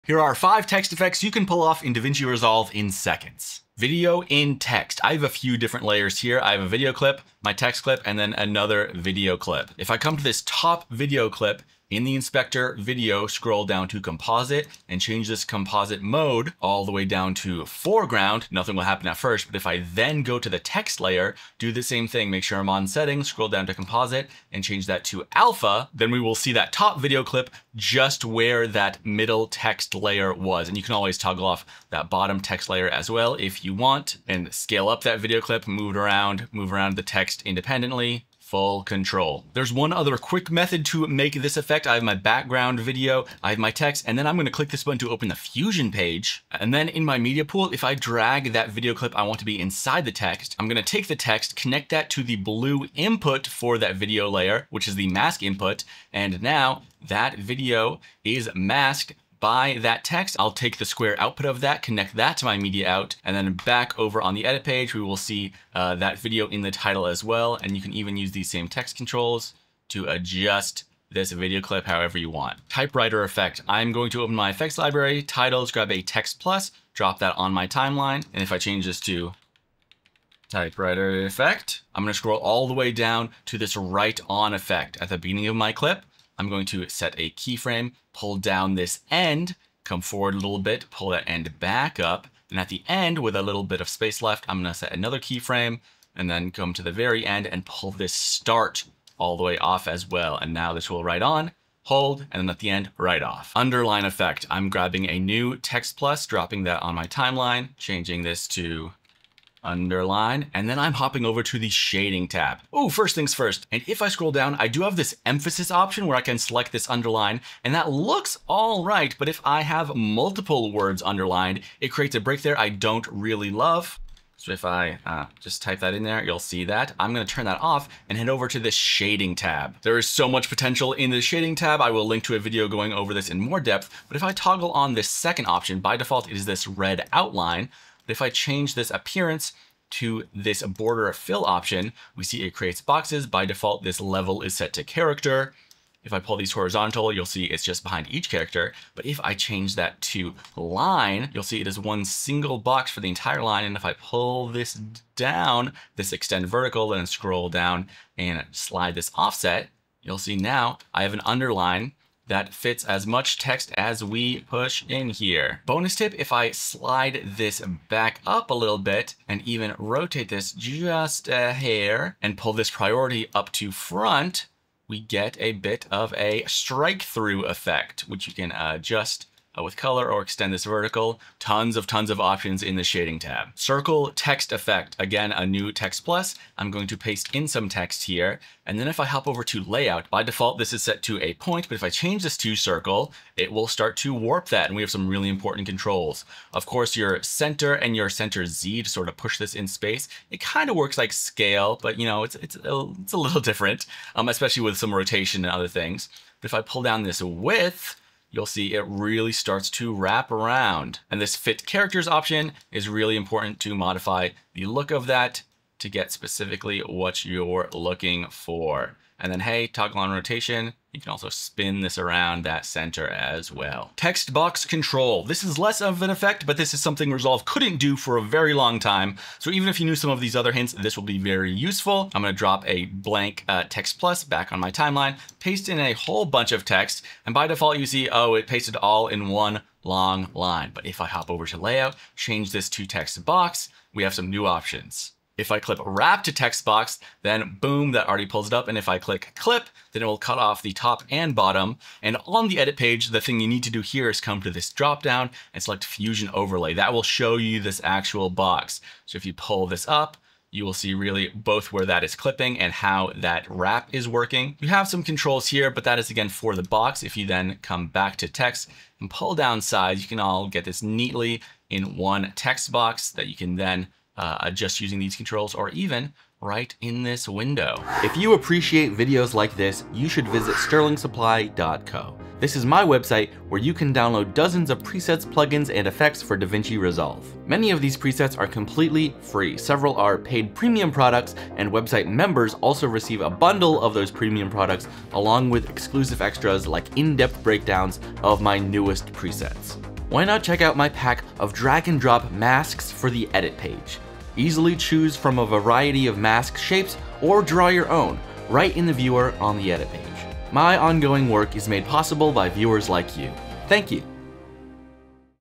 DaVinci Resolve Text Effects in SECONDS
00:00 - Five Fast Text Effects for DaVinci Resolve
00:03 - Video in Text Effect
01:12 - Typewriter Effect
01:43 - Underline Effect
03:03 - Circle Text Effect
03:43 - Text Box Control